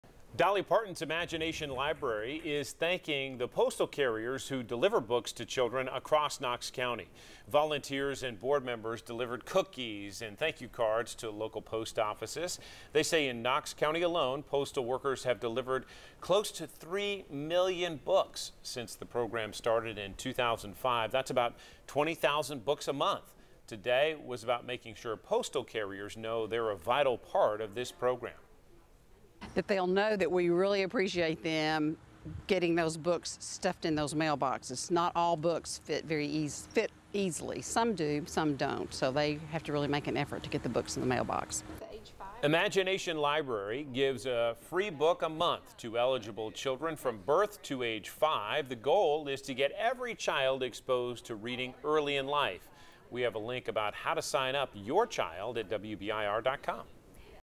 Dolly Parton's Imagination Library thanks postal workers
Dolly Parton's Imagination Library is thanking the postal carriers who deliver books to children across Knox County.